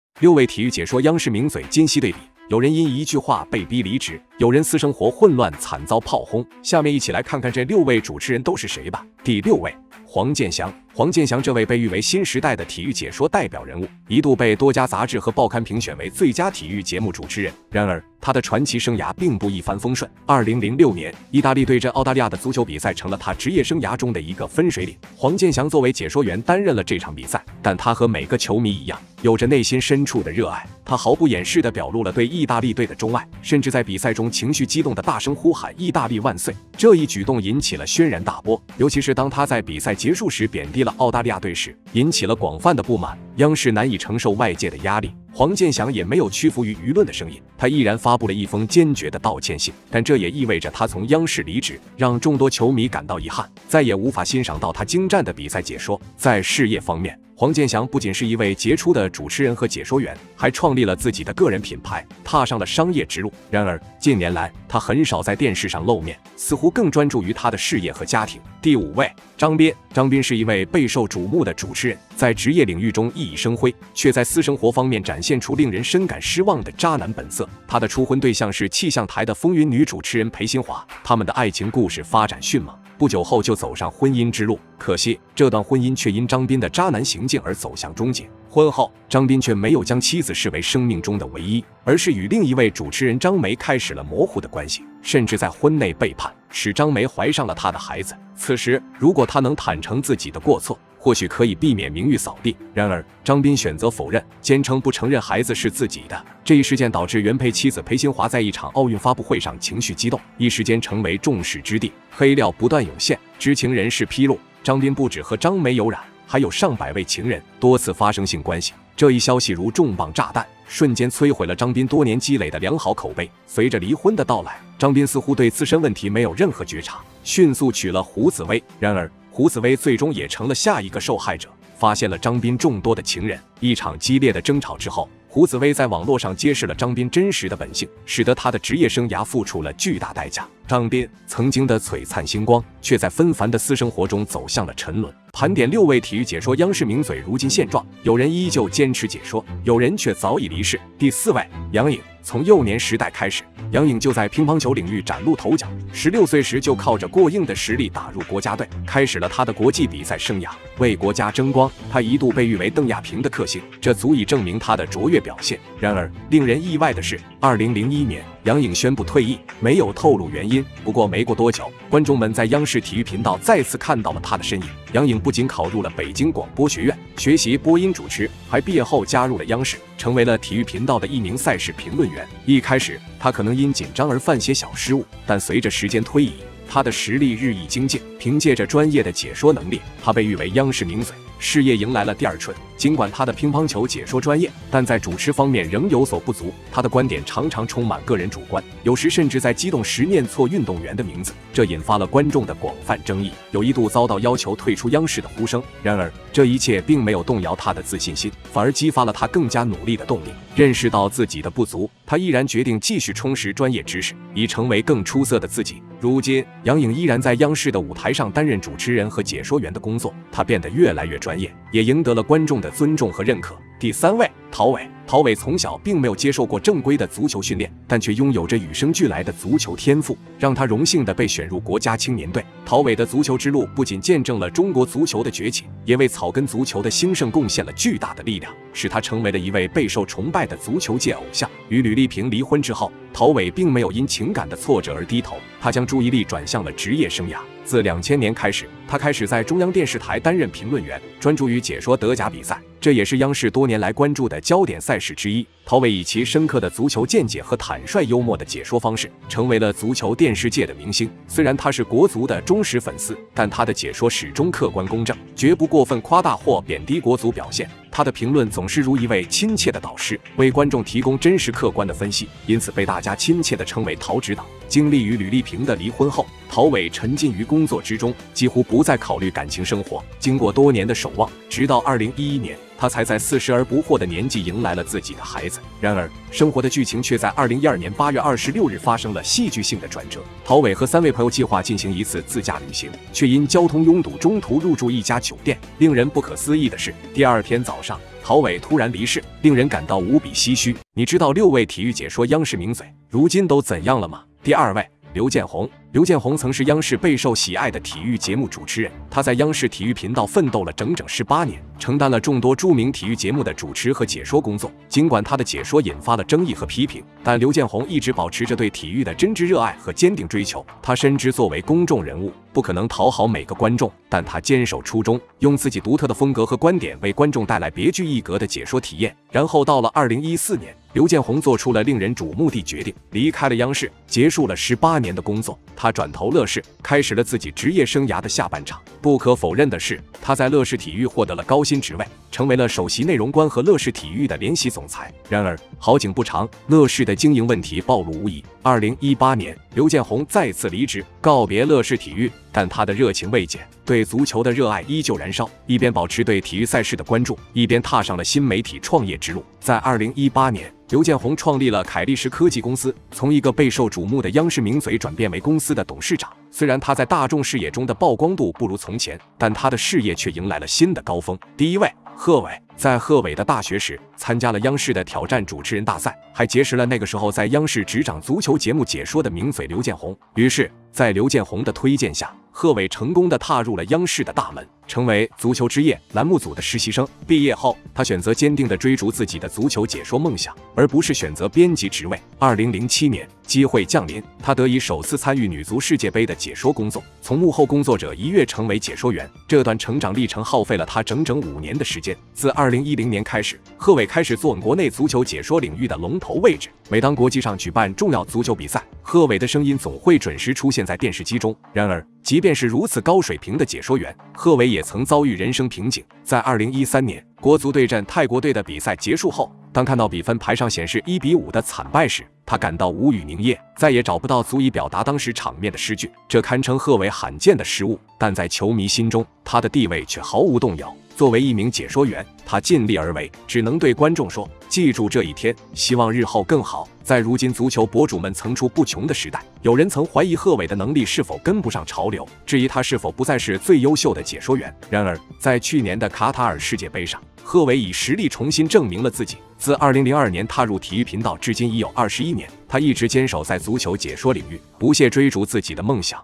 6位体育解说央视名嘴今昔对比，有人意外去世，有人遭全网炮轰
我吐槽明星，你们吐槽我#央视名嘴

欢迎留言吐槽、点赞、订阅。